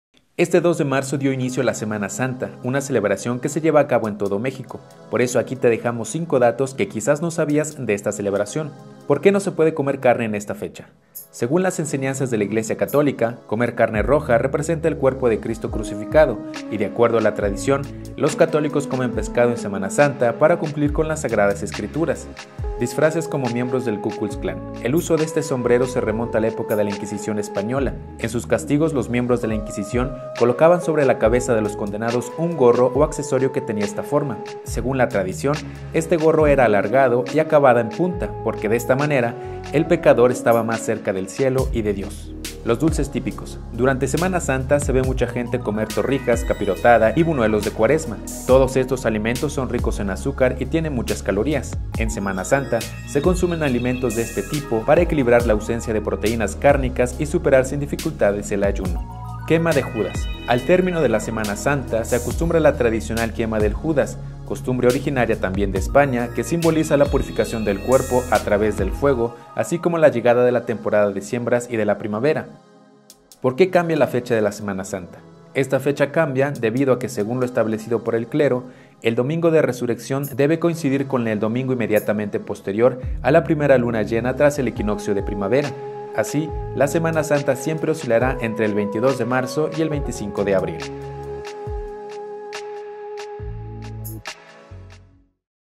5 datos que quizás no sabías de la Semana Santa
Este 2 de marzo dio inicio la SemanaSanta, una celebración que se lleva a cabo en todo México ✝️

Por eso aquí te dejamos 5 datos que quizás no sabías de esta celebración 🤔